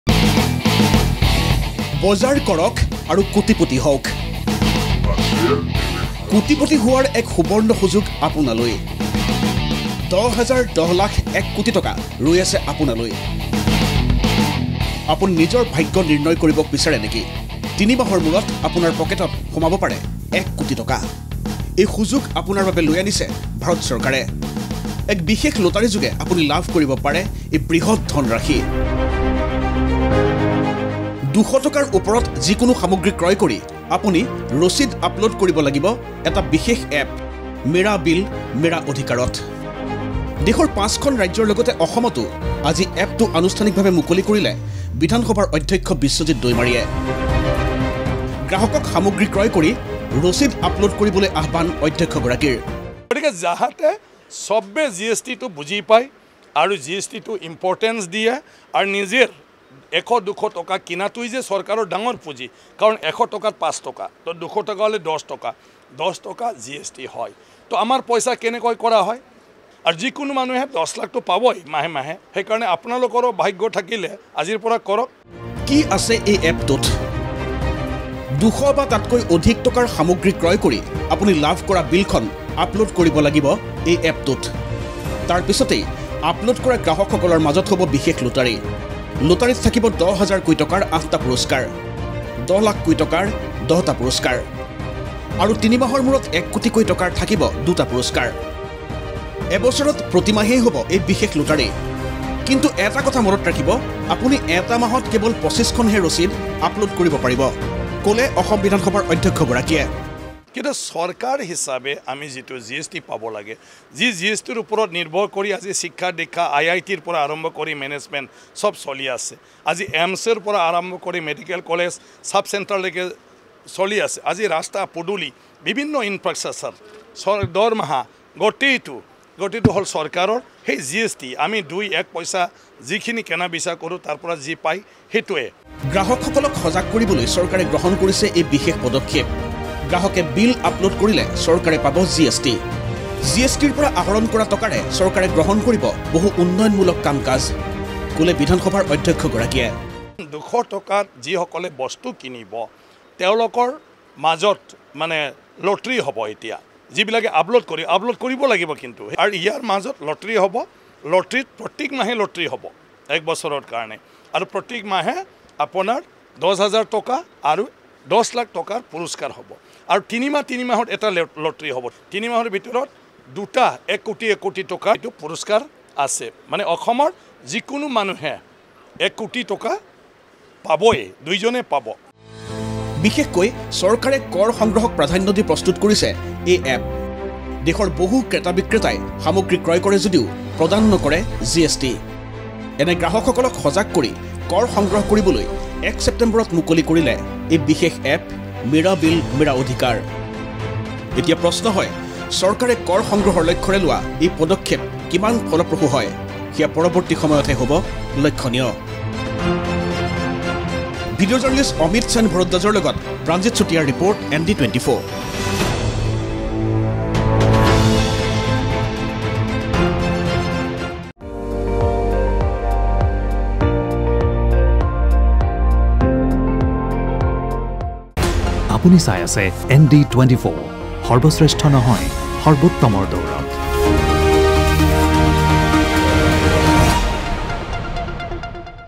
Mera Bill Mera Adhikar: গ্ৰাহকক সজাগ কৰিবলৈ চৰকাৰৰ বিশেষ পদক্ষেপ
গ্ৰাহকসকলক সজাগ কৰিবলৈ চৰকাৰে লৈছে বিশেষ পদক্ষেপ। মেৰা বিল, মেৰা অধিকাৰ শীৰ্ষক এপ মুকলি কৰি গ্ৰাহকলৈ আহ্বান বিধানসভাৰ অধ্যক্ষ বিশ্বজিত দৈমাৰীৰ। এপত বিল আপলোড কৰি আপুনি ৩ মাহৰ মূৰত লাভ কৰিব পাৰে ১ কোটি টকা।